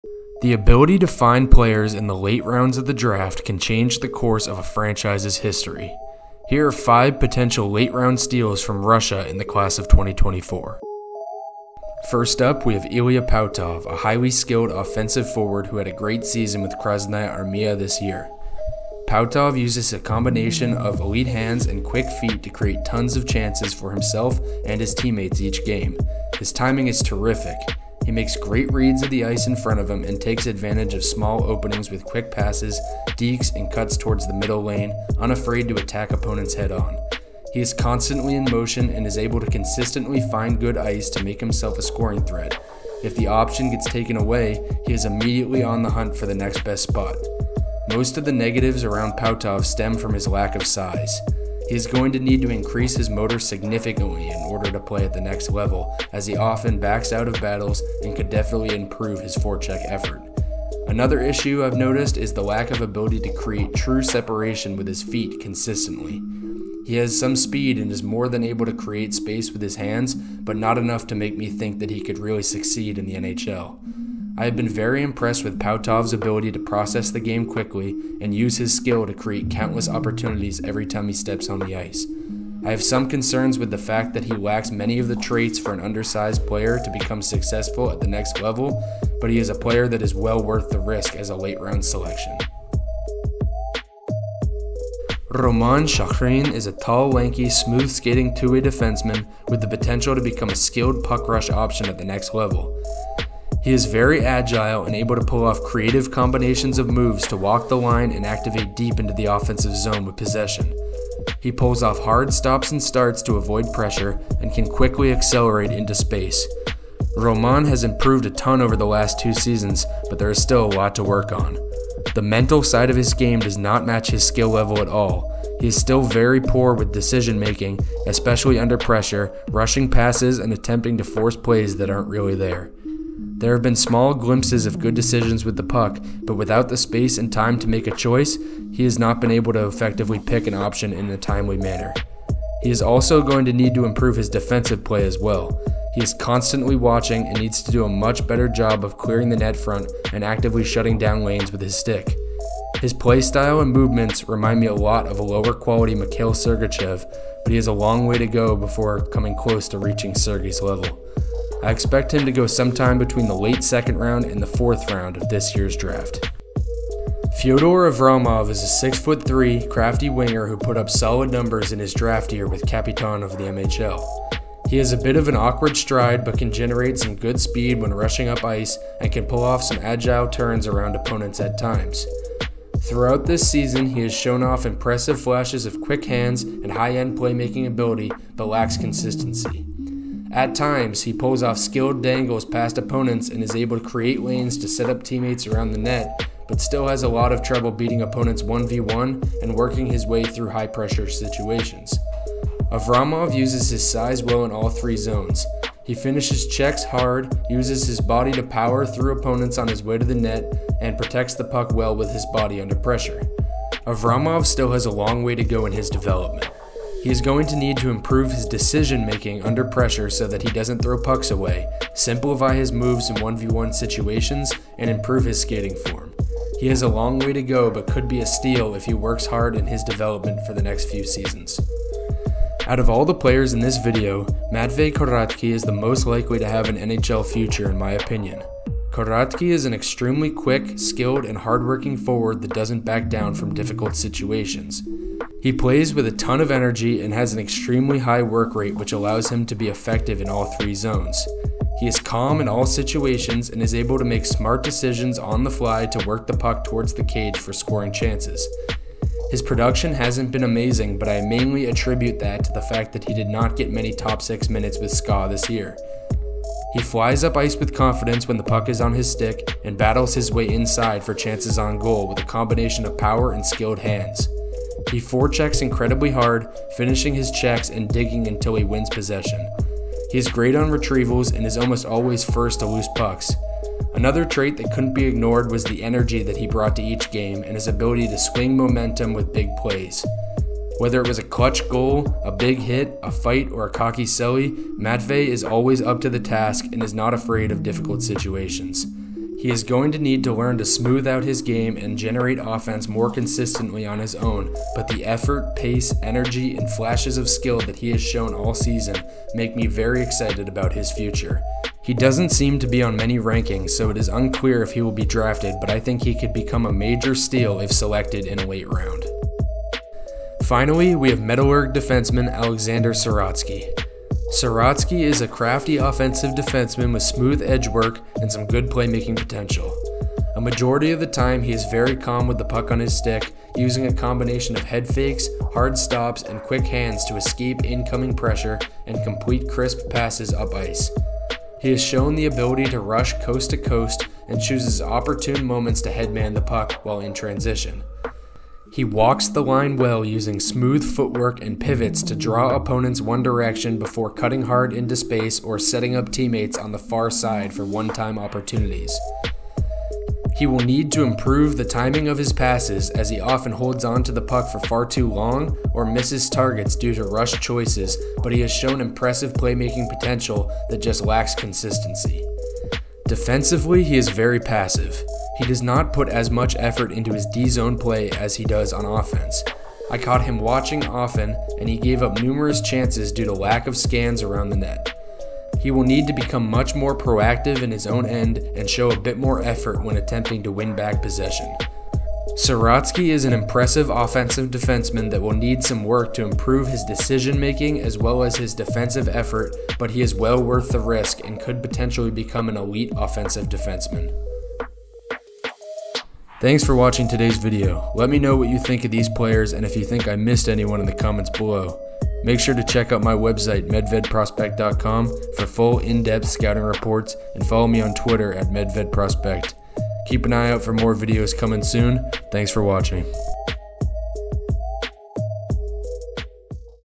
5 Potential Late Round Steals in the 2024 NHL Draft from Russia
5 Future Steals in the 2024 NHL Draft From Russia

These are some players that I feel are most underrated and underappreciated in the class of 2024 from Russia

Matvei Korotky, Ilya Pautov, Fyodor Avramov, Alexander Siryatsky, Roman Shokhrin

To read more about these players performances this year check out my